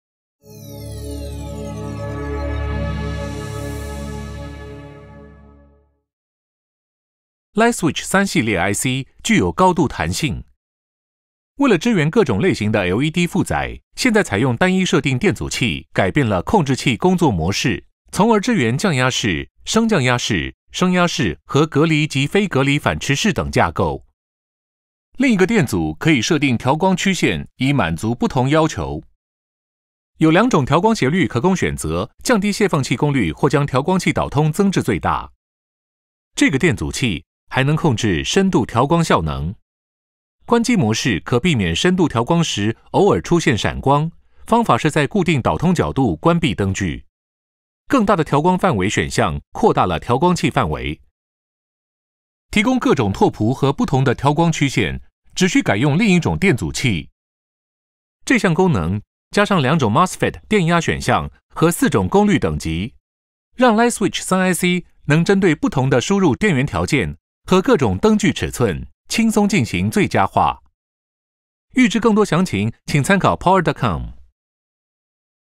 LYTSwitch-3 - Optimized Dimming Control - TW
LYTSwitch™-3 系列適用於 Single-stage 功率因數修正定電流 LED 燈泡和嵌燈。每部裝置都整合一個高壓功率 MOSFET 和不連續模式、可變頻率、可變開啟時間控制器。控制器還提供逐週期限電流 (cycle-by-cycle current limit)、輸出 OVP、線電壓過壓、全面的保護功能，以及進階散熱管理電路。